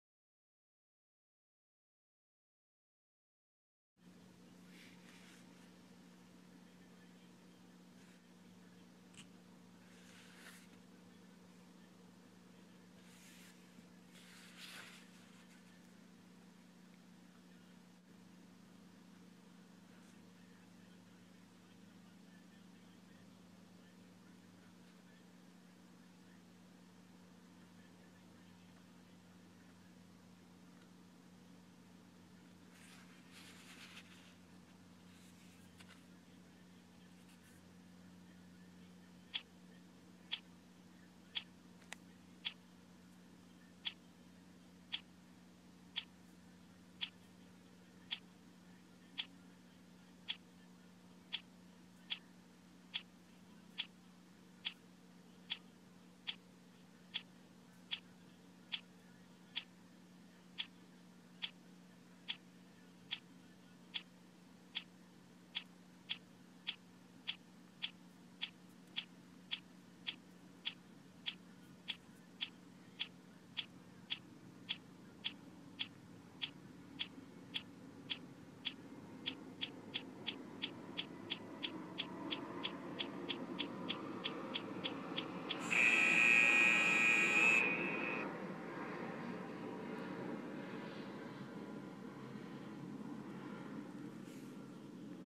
Catch Word
A great party game. Its like Hot Potato with words.